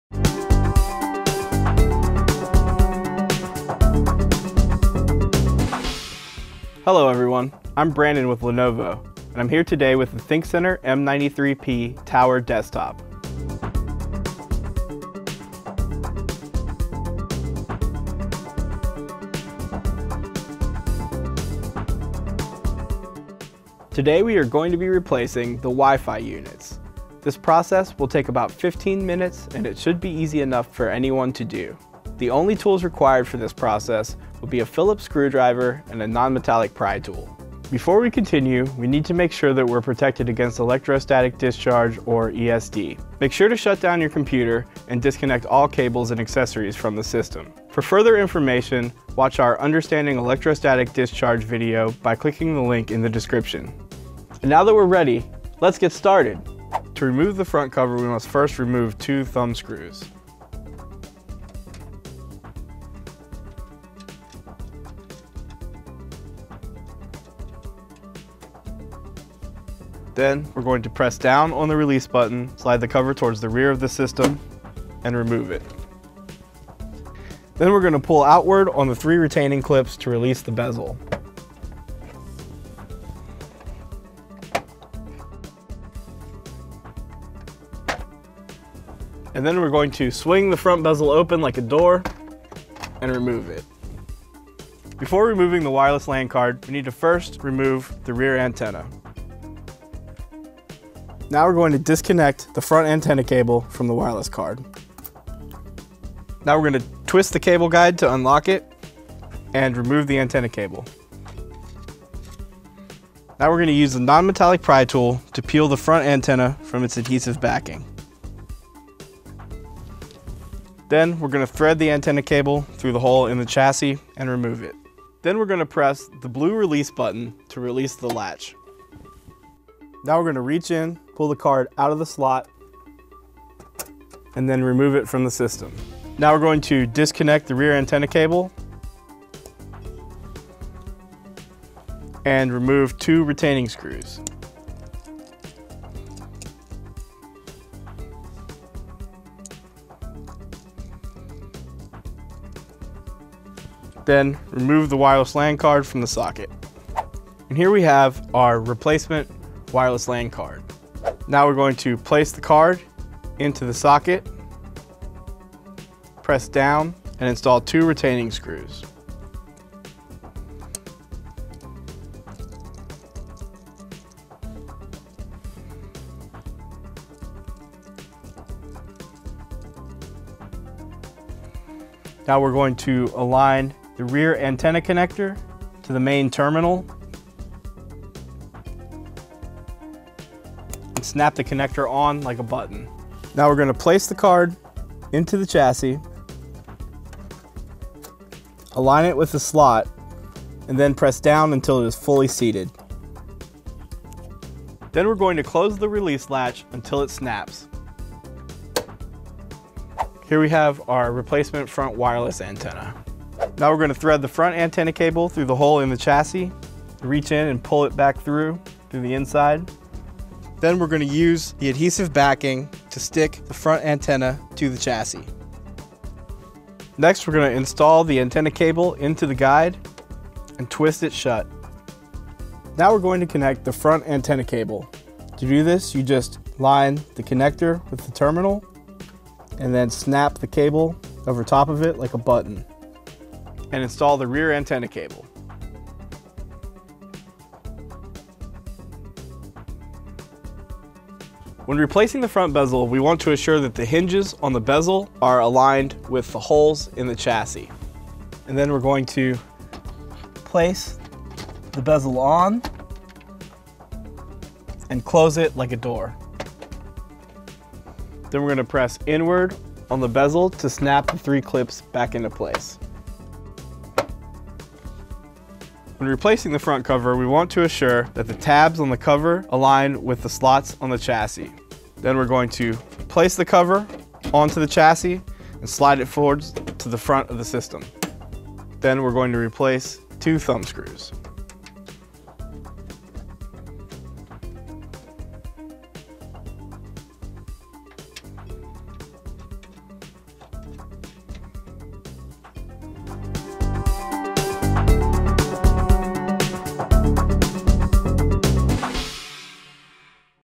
ThinkCentre M83 / M93p Tower Desktop - Wi Fi Units Replacement
This video will illustrate how to replace the Wi-Fi Units in your ThinkCentre M83 or M93p Tower Desktop.

Machine Types Applicable:
M83: 10A0, 10A1, 10A6, 10A7, 10AG, 10AK, 10AL, and
10BE
M93p: 10A0, 10A1, 10A6, 10A7, 10AG, 10AK, 10AL, and
10BE